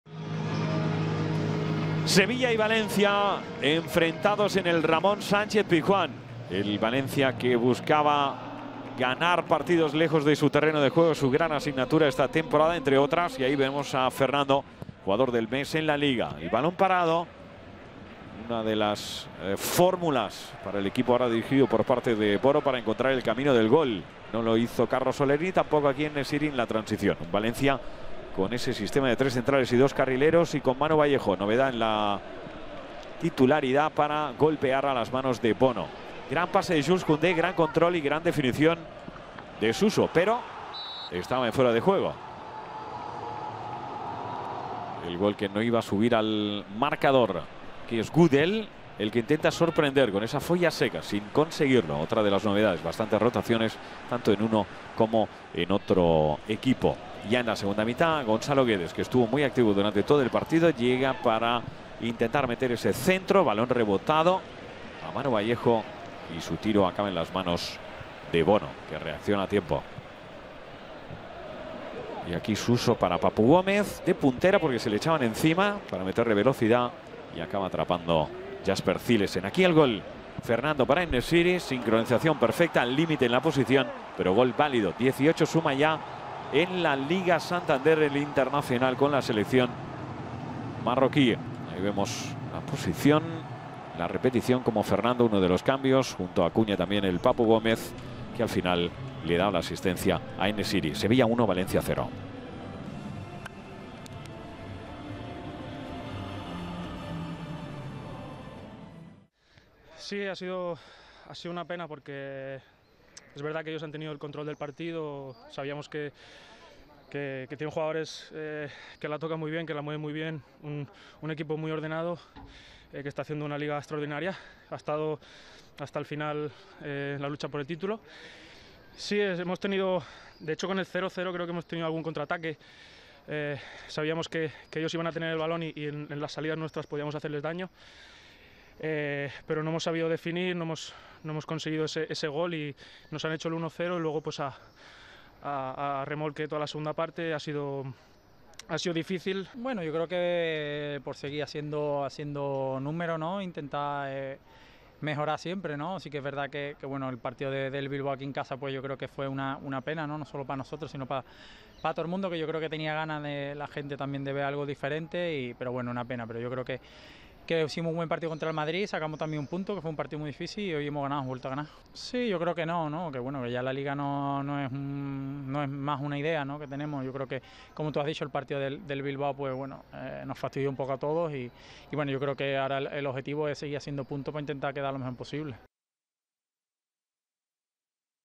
Resumen de Sevilla FC vs Valencia CF (1-0)
El Sevilla venció ante el Valencia CF con el decimoctavo gol en campeonato de Youssef En-Nesyri SevillaFCValencia J36 LaLiga Santander 2020/2021